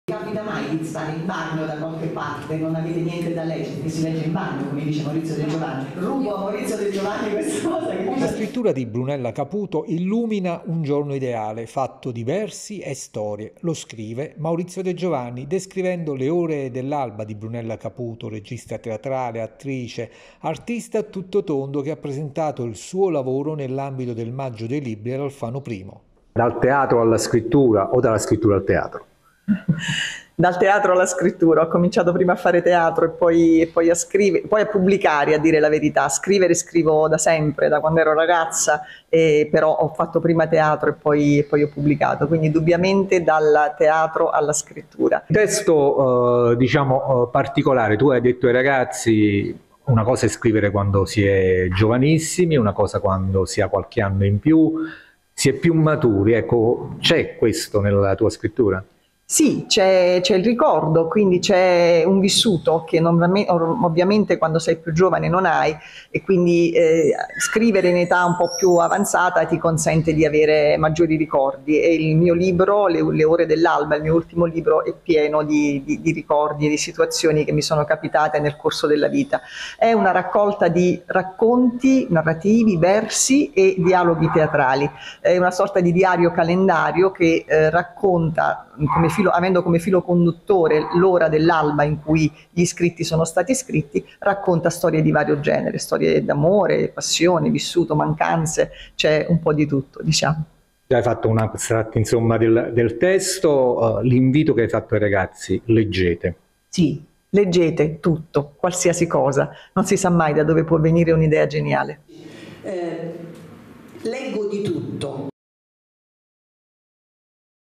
Scrittori salernitani al maggio dei libri con Brunela Caputo e "Le ore dell'alba"
"Le ore dell'alba", al maggio dei libri all'Alfano I° a Salerno il libro  di Brunella Caputo